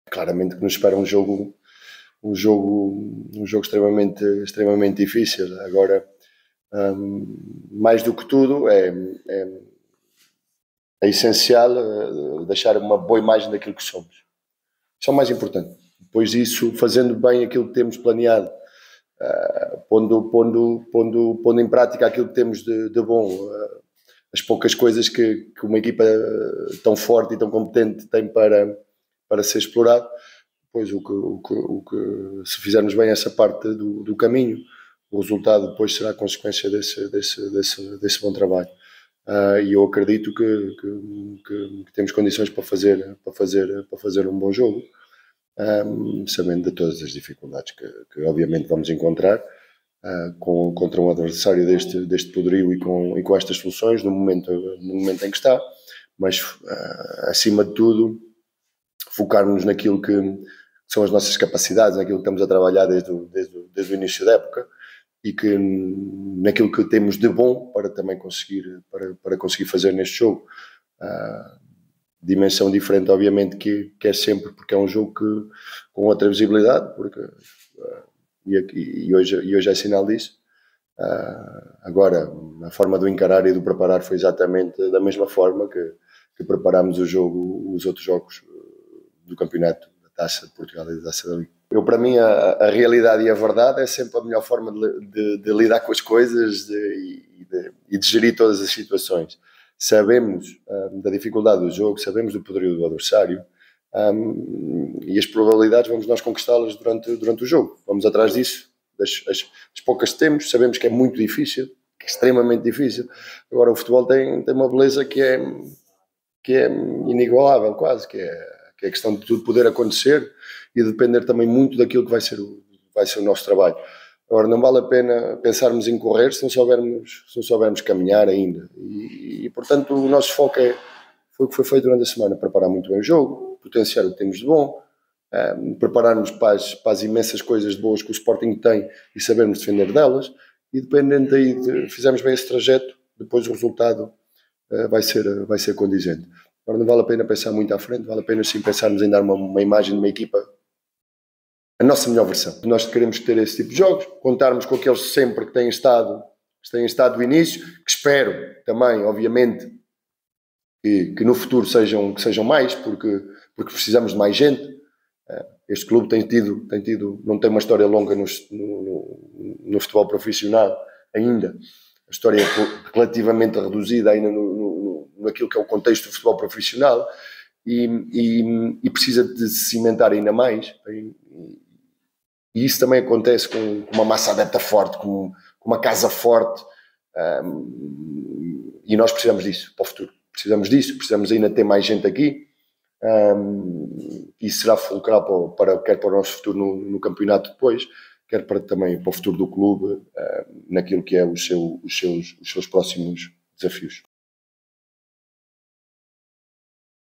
Tozé Marreco | "Temos condições para fazer um bom jogo"
O Mister Tozé Marreco fez esta manhã a antevisão ao duelo frente ao Sporting CP 👊🔰